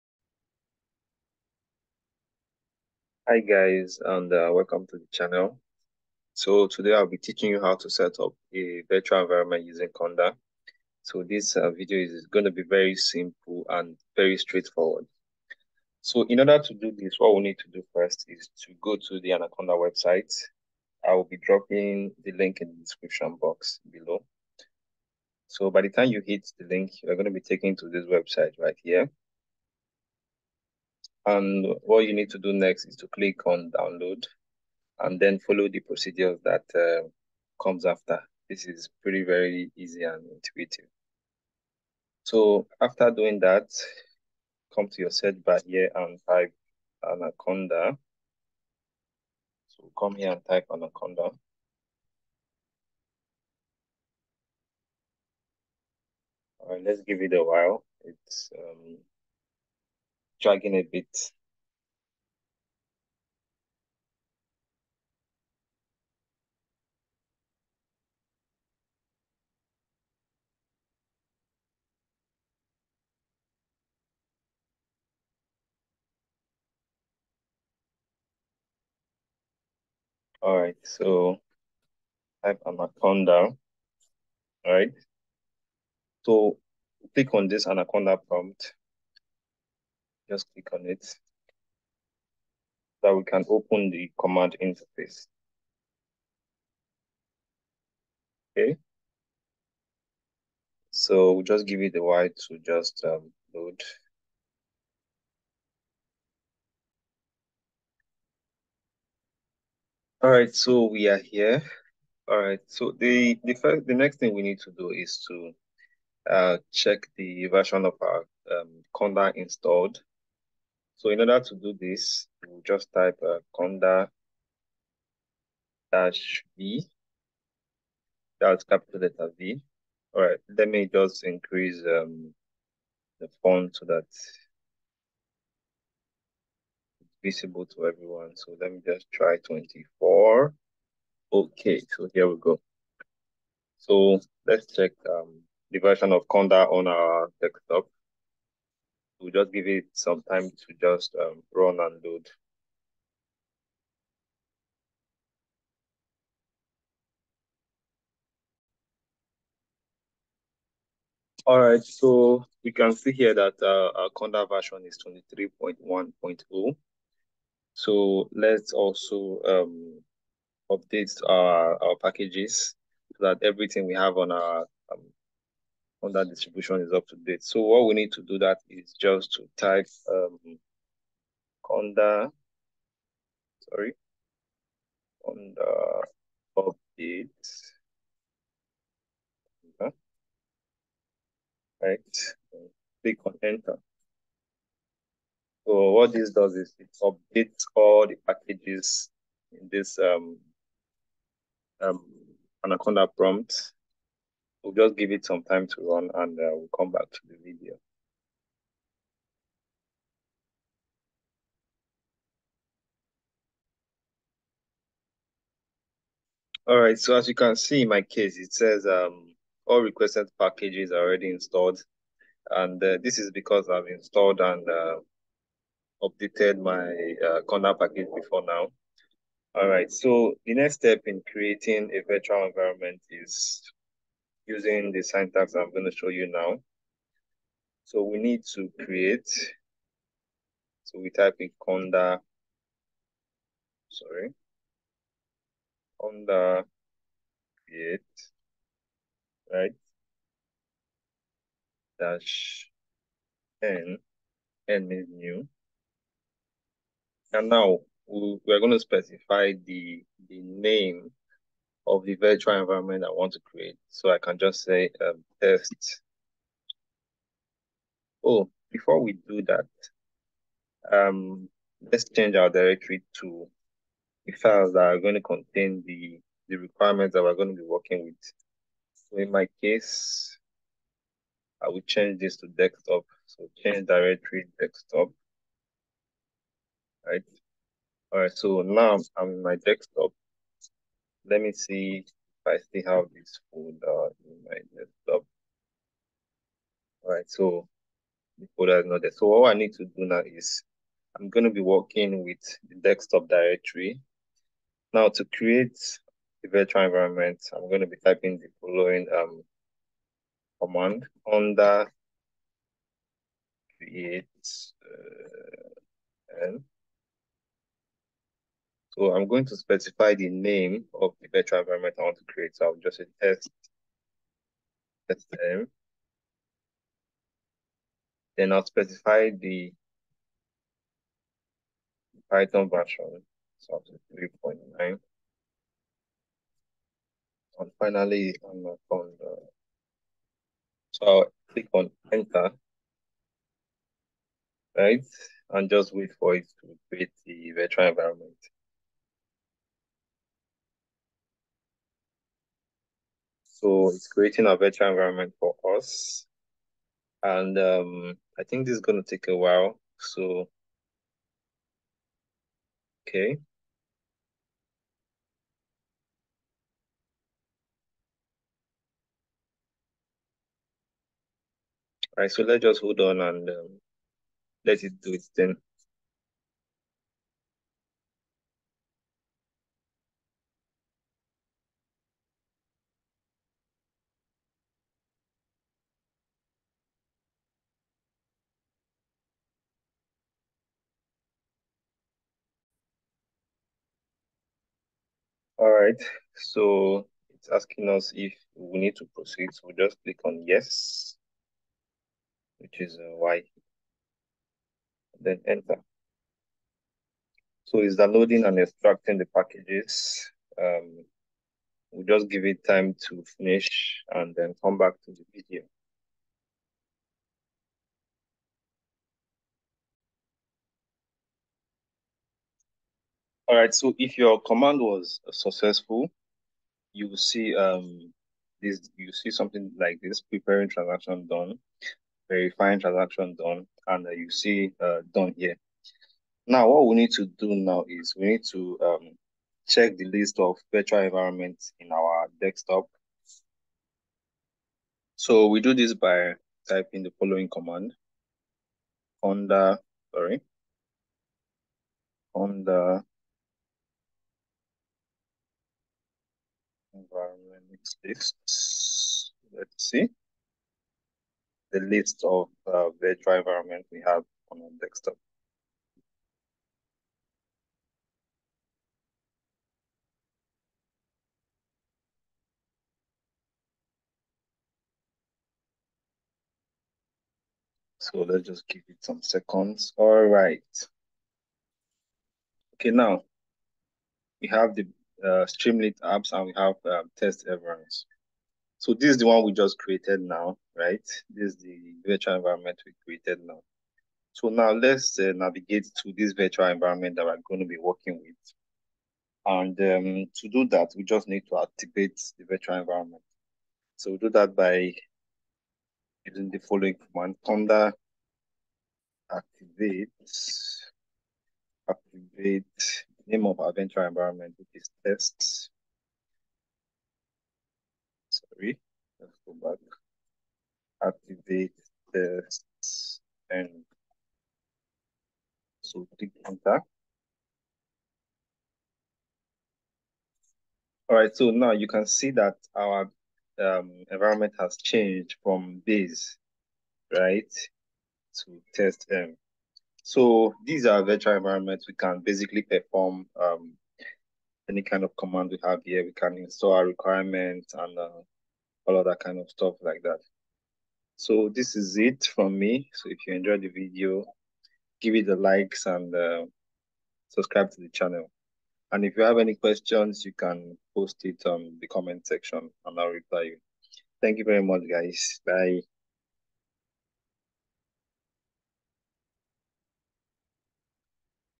How to Setup a Virtual Environment Using Conda.
This video explains how to set up a virtual environment using Conda.

how to set up virtual environment using conda